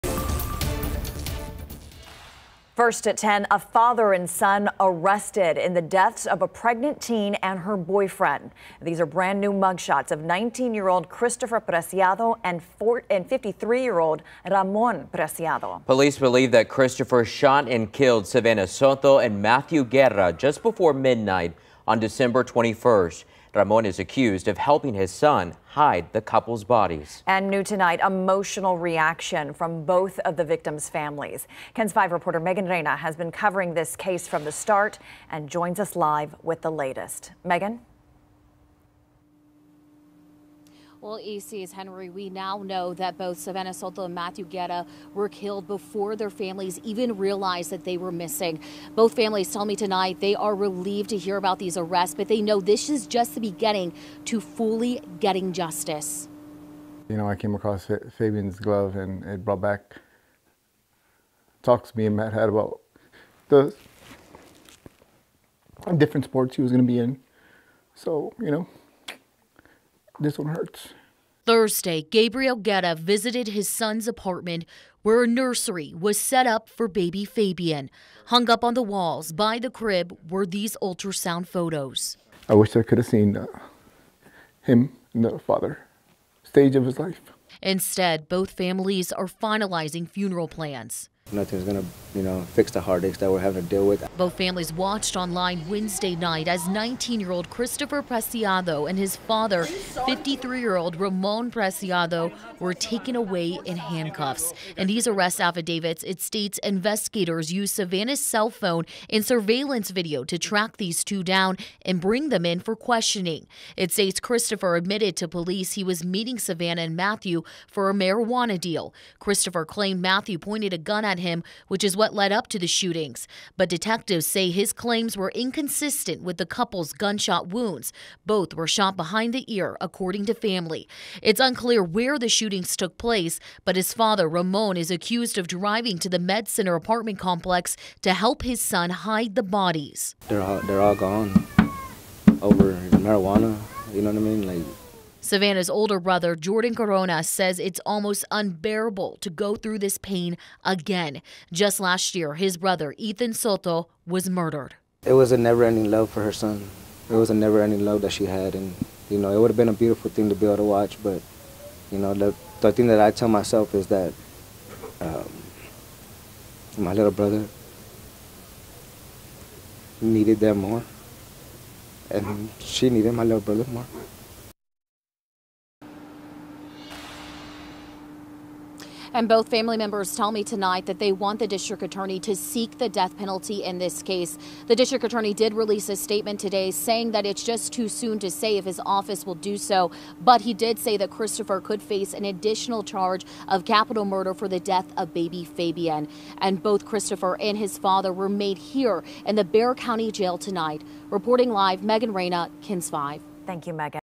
Soto and Guerra families react to accused killer being arrested
A 19-year-old San Antonio man was arrested on capital murder charges, and his father also taken into custody, eight days after the bodies of a pregnant teen and her boyfriend were found. Savanah Soto was scheduled to have her labor induced two days after police say she was murdered.